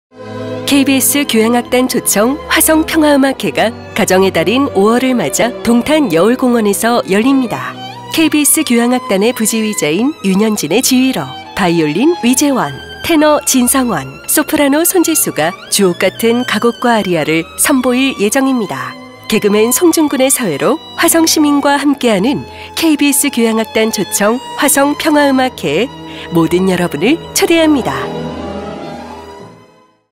KBS교향악단 초청 ㅣ 화성 평화음악회
가정의 달 5월을 맞이하여 동탄여울공원에서

KBS교향악단을 초청

주옥같은 가곡과 아리아를 선보일

화성 평화음악회~ 많은 참여 바랍니다.